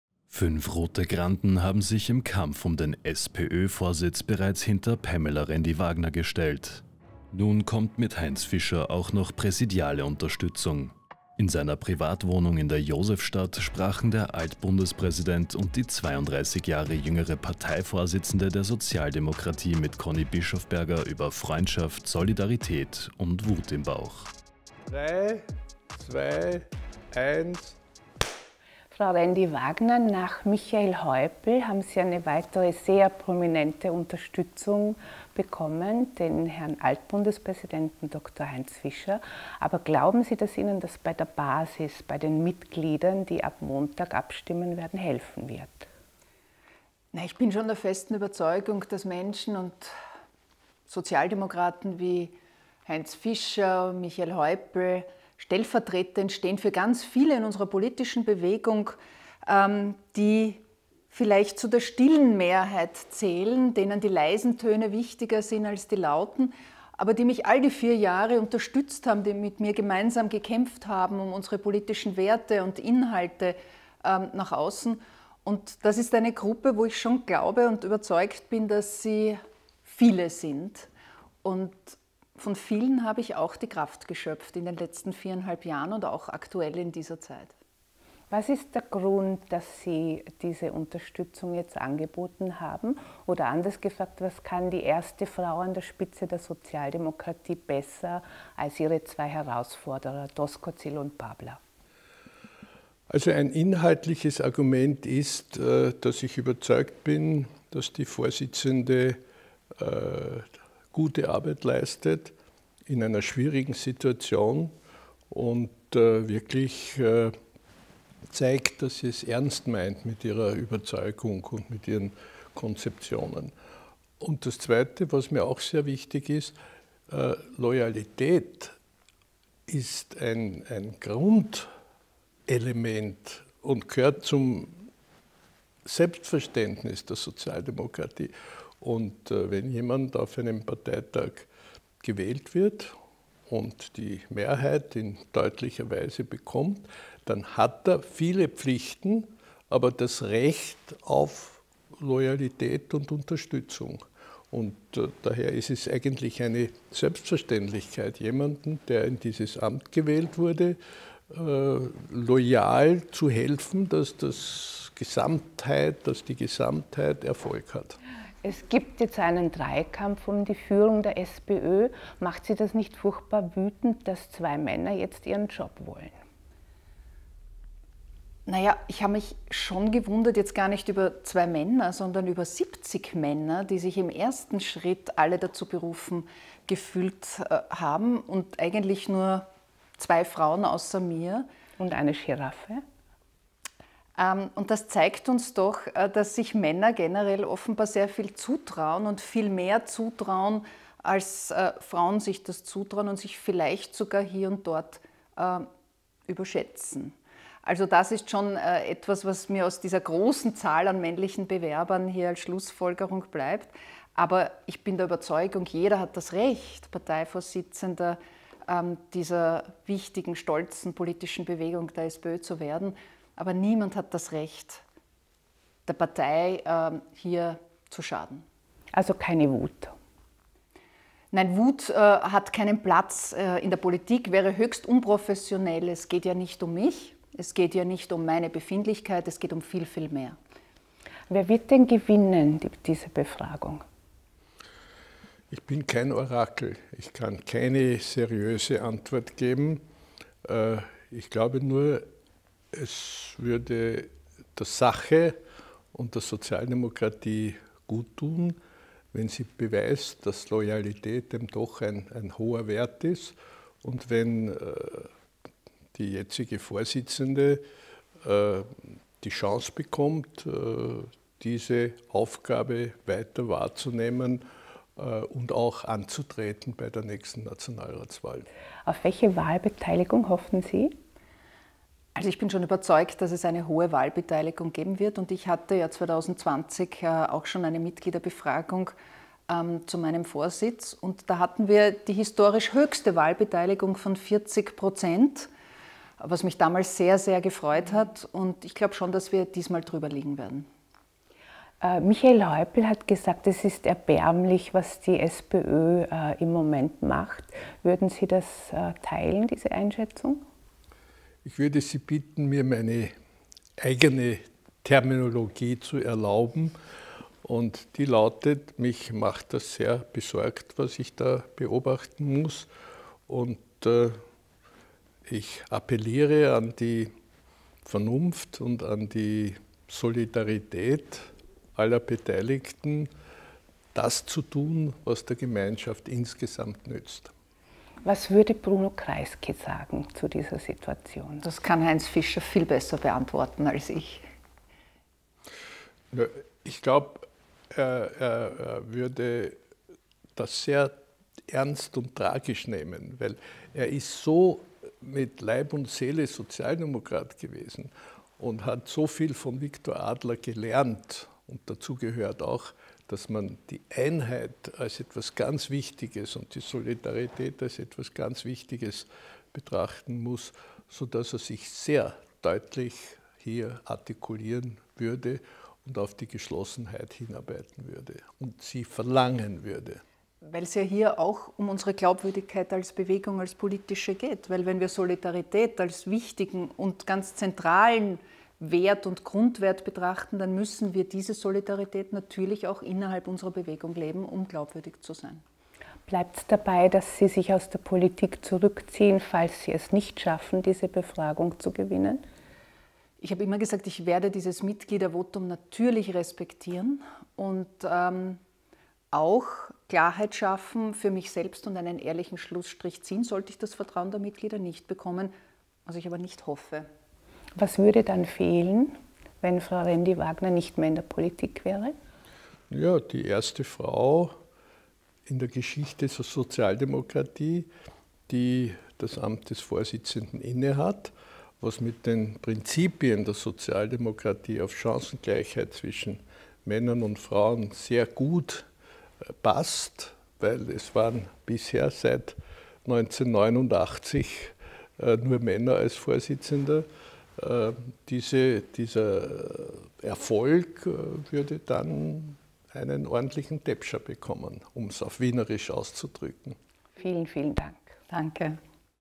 Auch Heinz Fischer unterstützt Rendi-Wagner | NEWS krone.tv
Fünf rote Granden haben sich im Kampf um den SPÖ-Vorsitz bereits hinter Pamela Rendi-Wagner gestellt. Nun kommt mit Heinz Fischer auch noch präsidiale Unterstützung. Ein Doppelinterview über Freundschaft, Solidarität und Wut im Bauch.